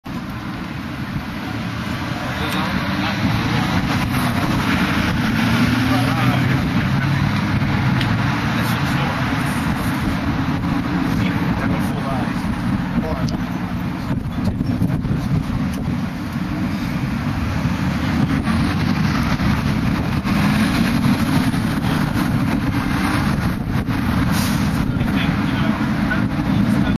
4:30am 3G iPhone Queue - Sydney Australia
waiting for the Telstra shop to open, which is across the road from the Apple store. Apple junkies slept out